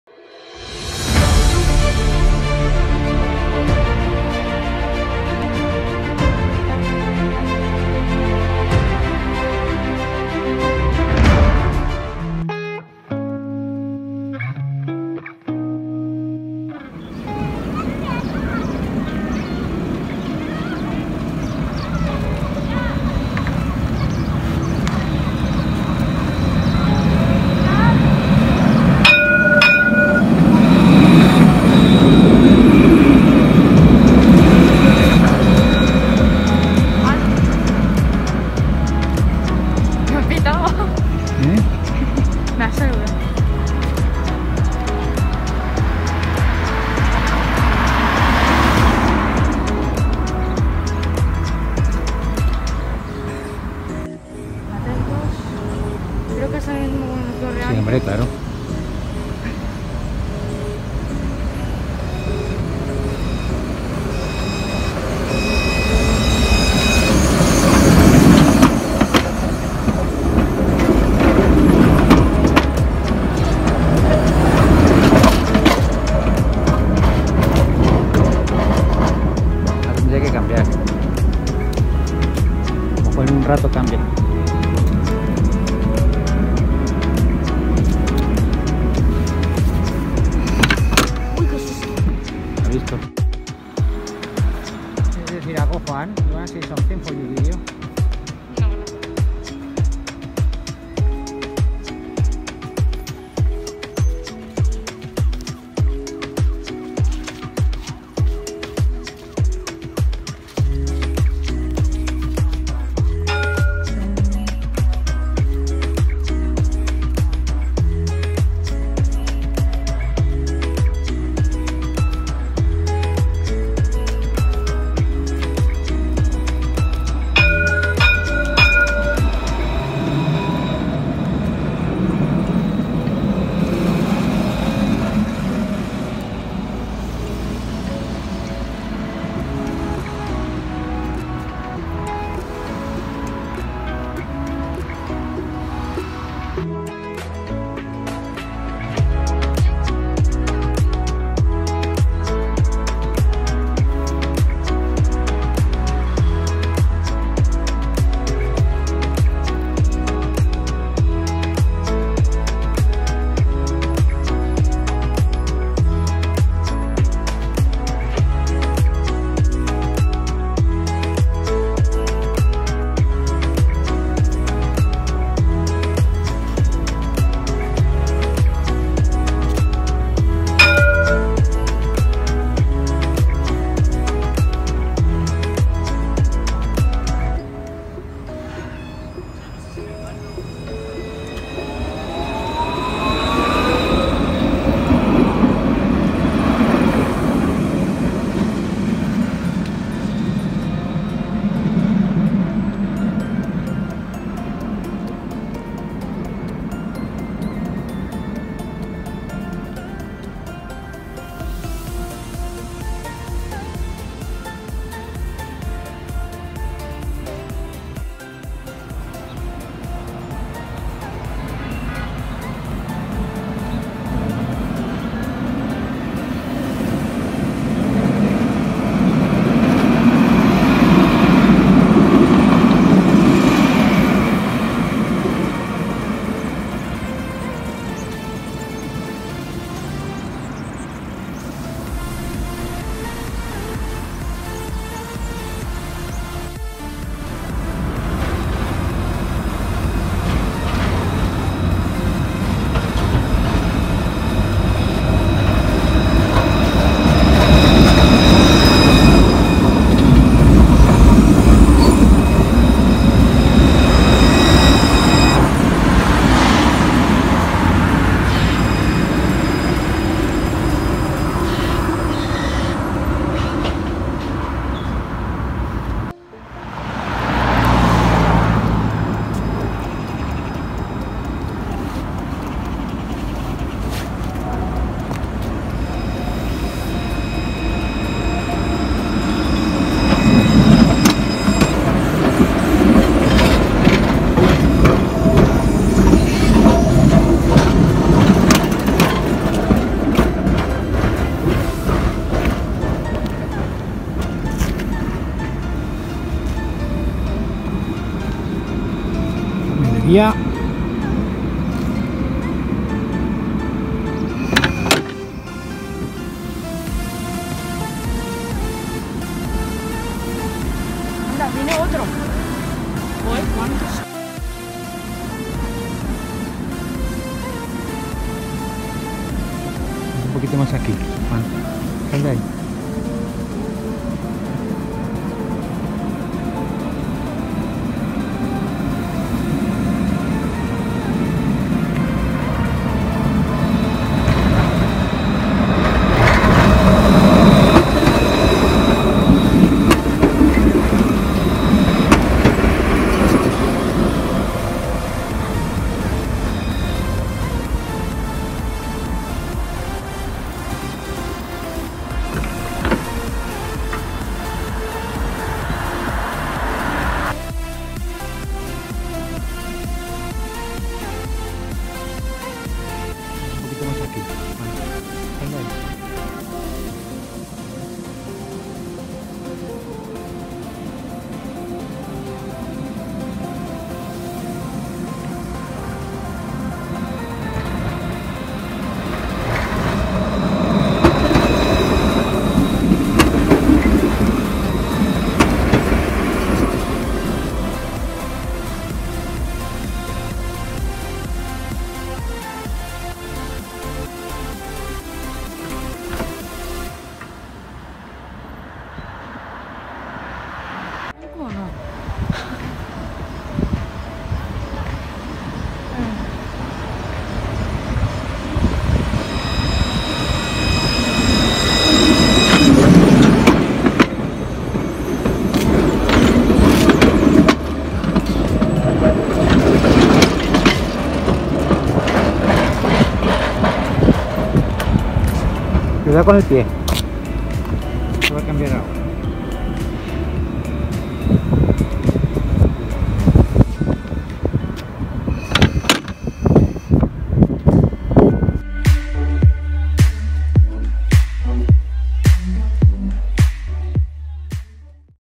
Línea Metro Ligero ML1 - Las Tablas
Vemos tranvías llegando y saliendo de la estación Las Tablas en la linea ML1